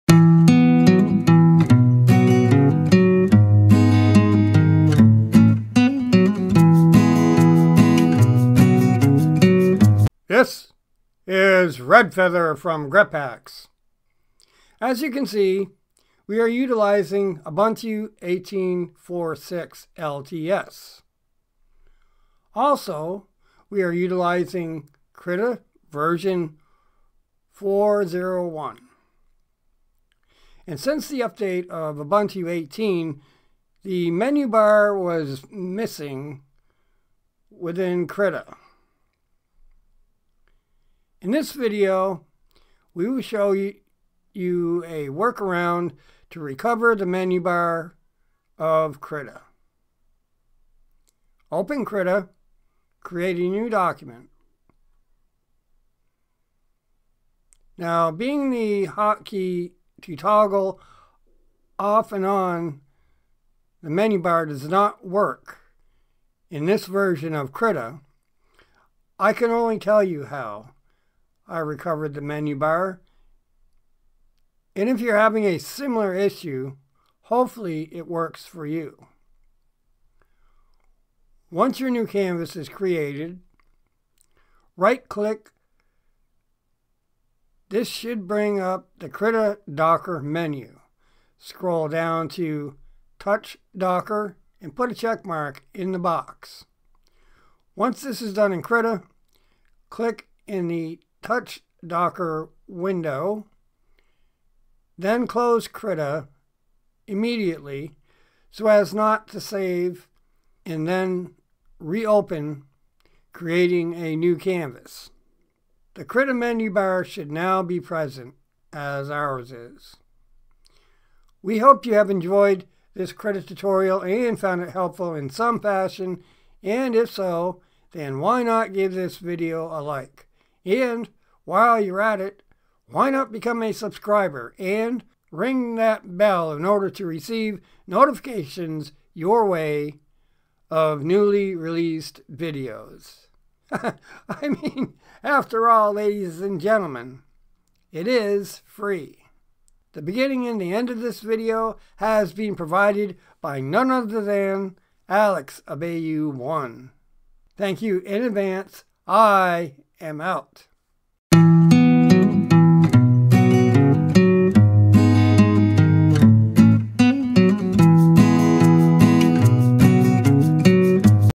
Krita How To Recover Menu Bar Version 4 0 1
In this Krita video we show how to recover the menu bar of
Krita version 4.0.1 in Ubuntu 18.04.6 LTS . We explain this
occurred during the update to Ubuntu 18 and that this is a
workaround . In order to do this we utilize the Krita touch docker
function of Krita . Closing immediately after clicking in the
touch docker window so as not to save any function other
than the recovery of the Krita menu bar .

0:00 Intro
0:11 Explanation of Krita video
0:40 Showing Krita missing menu bar
1:32 Showing how to bring up docker menu
2:04 Clicking in touch docker window so as to recover menu bar
2:41 Outro